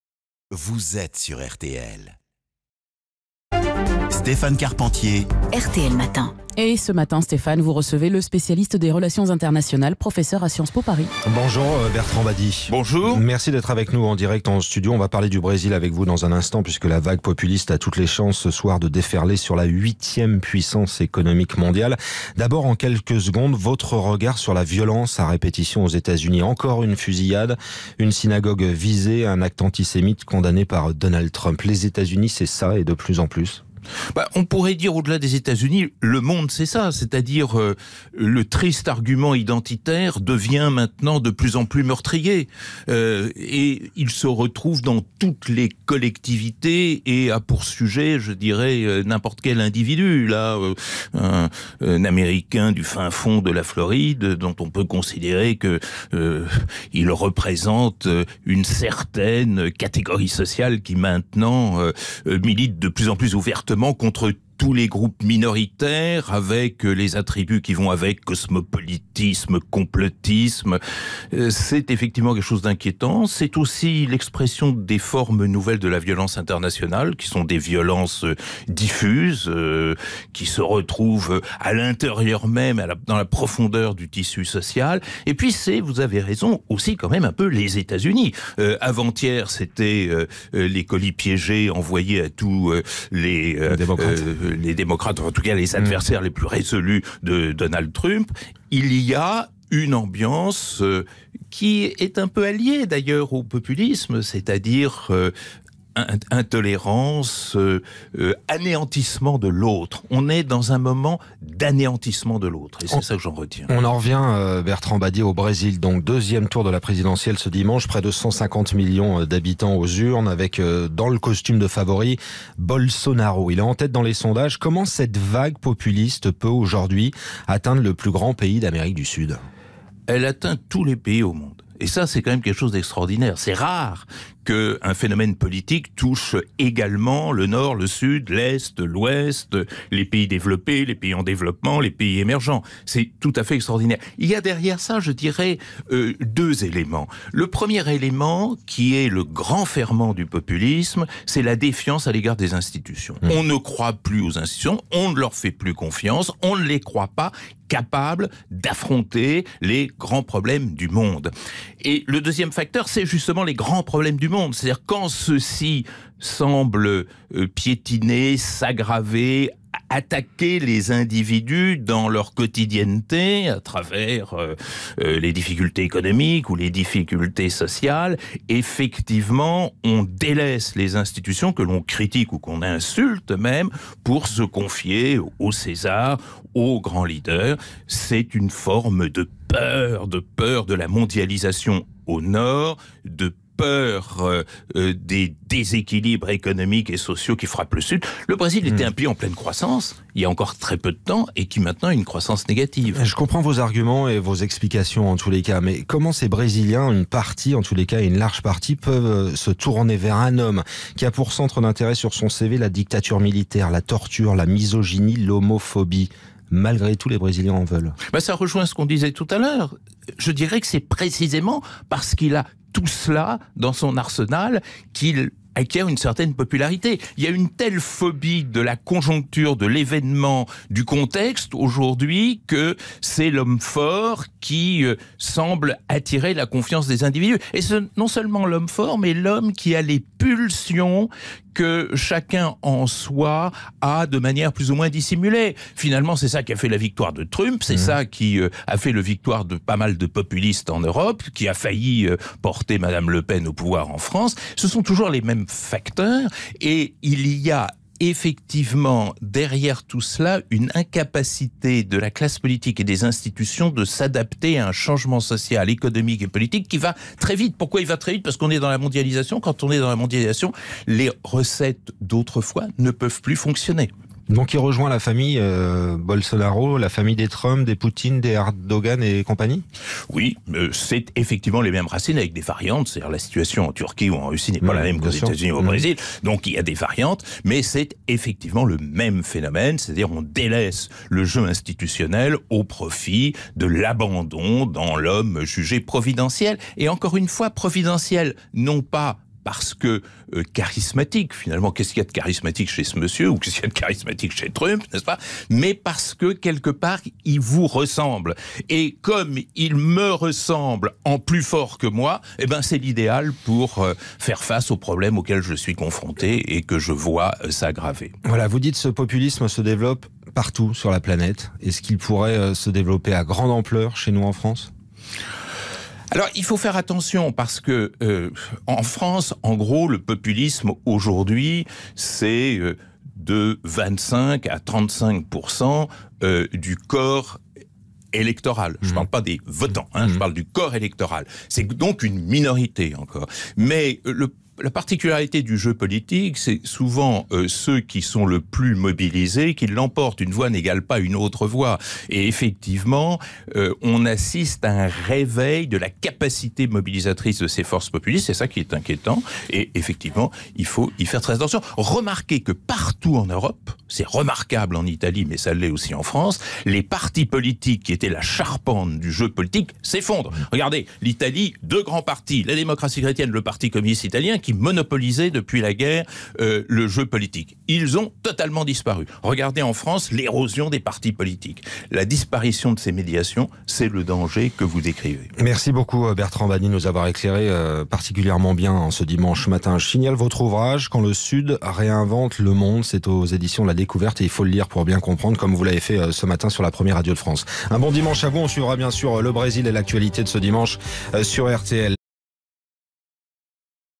Brésil : "Bolsonaro, c'est les mêmes racines que Trump, Poutine et Erdogan", dit un spécialiste
INVITÉ RTL - Bertrand Badie, professeur à Sciences Po Paris, revient sur la vague populiste qui menace le Brésil, à quelques heures du second tour de l'élection présidentielle.